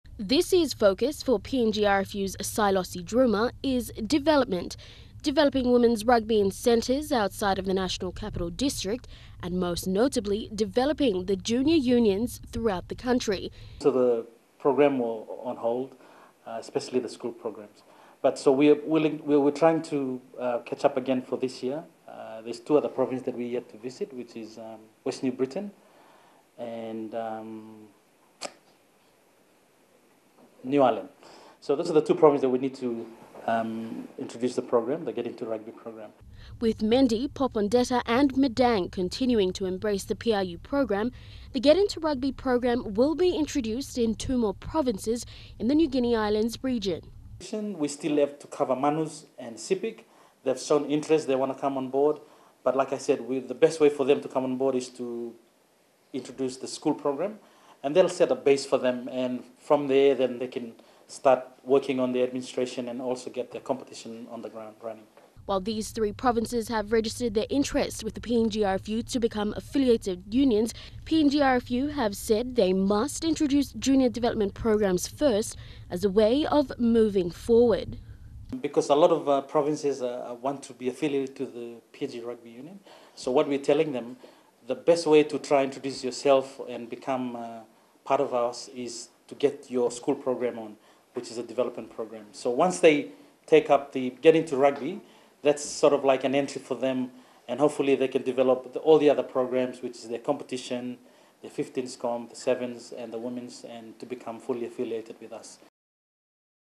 Get Into Rugby Program To Expand
The Get into Rugby program, has been the driving force for the growth of Rugby Union in the Pacific.

Similar to the Pacific in Union curriculum based program, GiR is also a school based program which focuses more on the on-field practices of rugby.

While many of the rugby programs were put on hold in 2015 due to the 2015 Pacific Games and other events held by the PNGRFU, this year will see the resumption and expansion of many of these programs, to the outer centres.